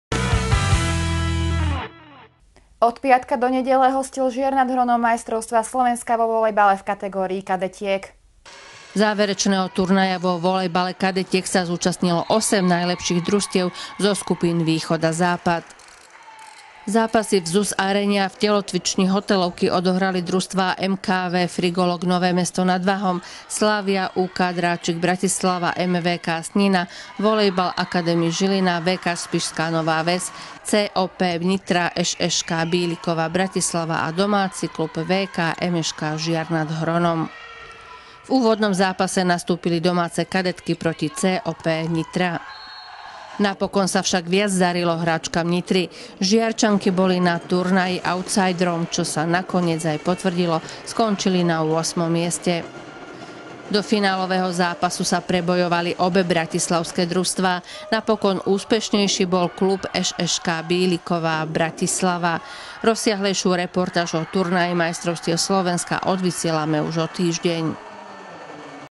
Majstrovstvá Slovenska vo volejbale boli v Žiari
Od piatka do nedele hostil Žiar nad Hronom  Majstrovstvá Slovenska vo volejbale v kategórii kadetiek.